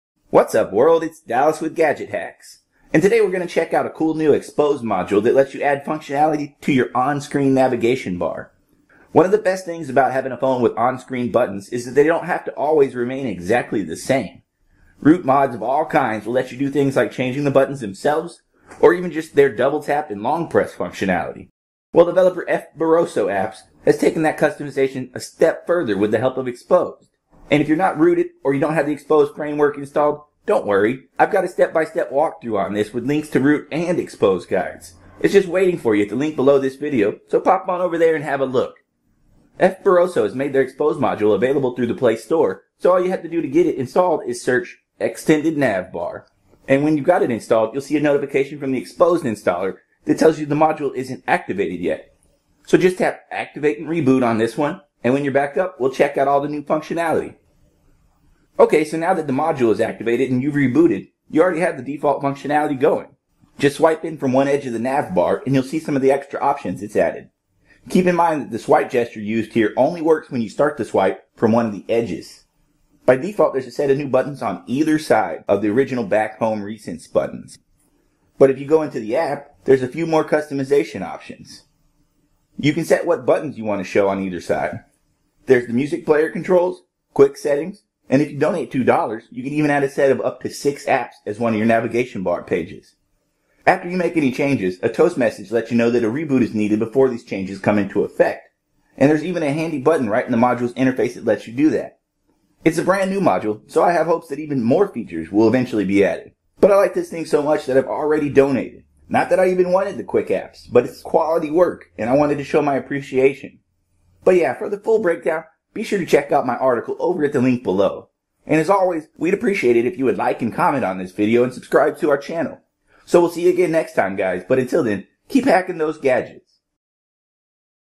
Add More Options to Your On-Screen Buttons - Nexus 5 or Any Rooted Android [How-To]
How to Add More Functionality to Your Navigation Bar

In this tutorial, I'll be showing you how to add Quick Settings and even music player controls to the navigation bar that's in use by almost every current-generation flagship Android device. I'll be demonstrating this on my Nexus 5.

Instead of the tired old "Back-Home-Recents" taking up all the glory at the bottom of your screen, you can easily swipe between different panels to access more controls on any screen.

This requires root and the Xposed Framework, and tutorials on those are available at the link above.

For more information and for downloads, check out the full tutorial over on Gadget Hacks using the link above.